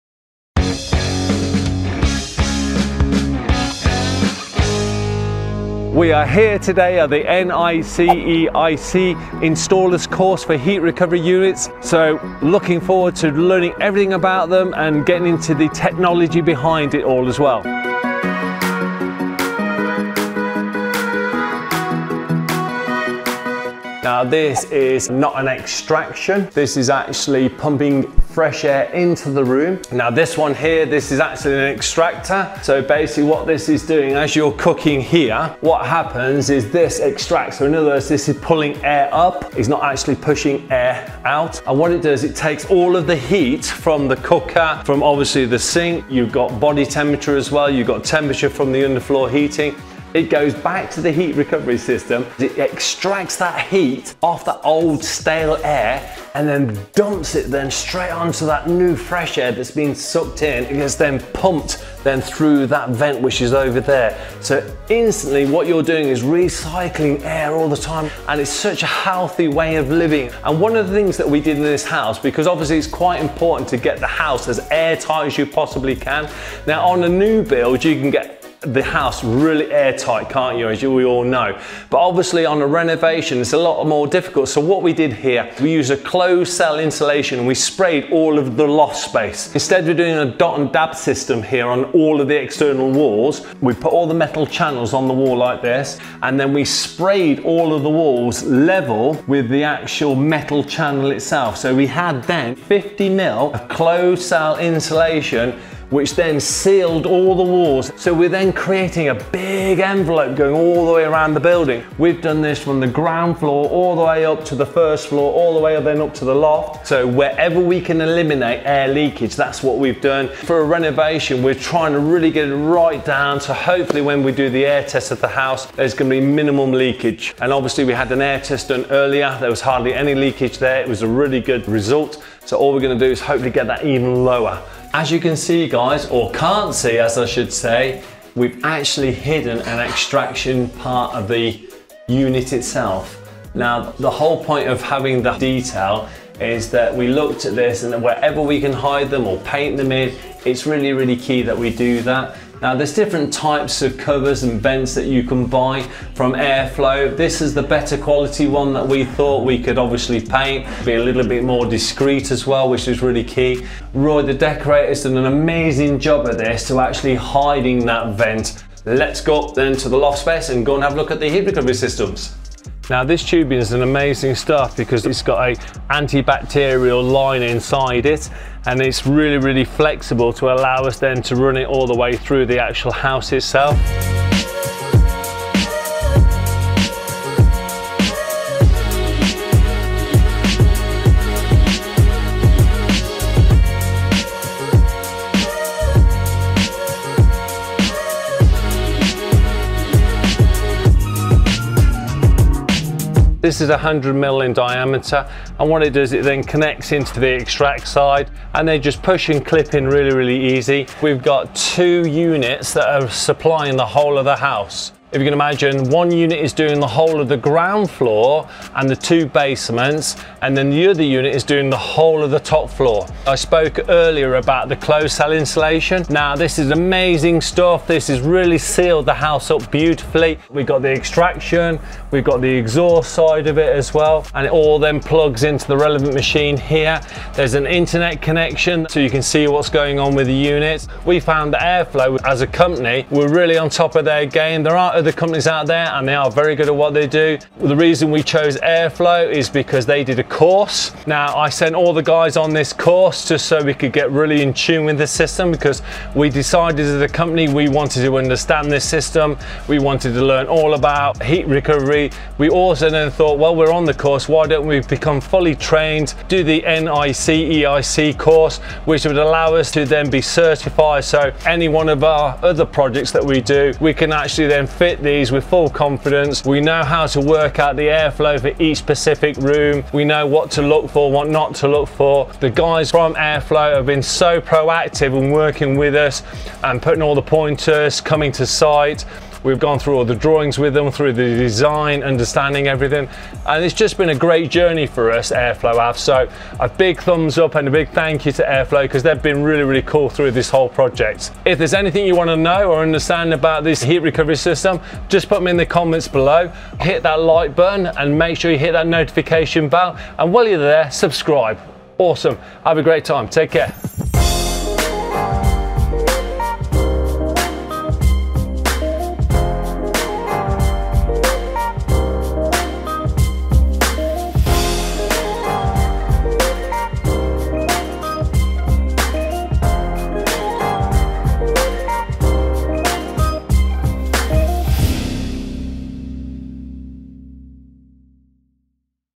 Installing a Heat Recovery and Ventilation System
In this video Tony explains Installing a Heat Recovery and Ventilation System in one of our extension build projects. Tony explains what is MVHR and how the whole house was wrapped in closed cell insulation to make it air tight prior to installing the MVHR. The system installed was an Airflow DV245.

PLEASE NOTE: we’ll endeavour to answer questions and comments as soon as possible depending on our work schedules.